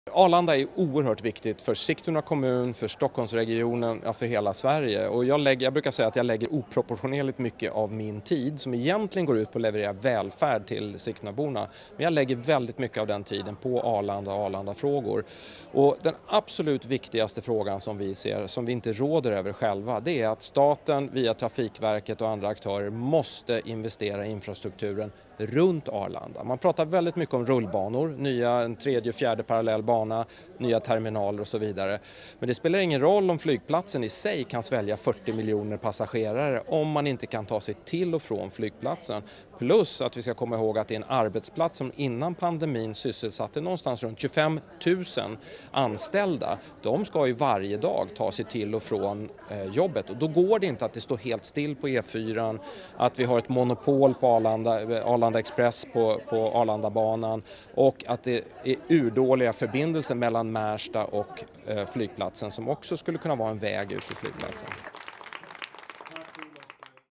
Arlandadagen 2021: Måste satsa på infrastrukturen runt Arlanda
-Det pratas väldigt mycket om rullbanor och nya terminaler men det spelar ingen roll om flygplatsen kan svälja 40 miljoner passagerare om man inte kan ta sig till och från flygplatsen, sa Olov Holst, kommunstyrelsens ordförande (M) i Sigtuna kommun, då Stockholms Handelskammare tillsammans med Airport City Stockholm arrangerade Arlandadagen i november 2021.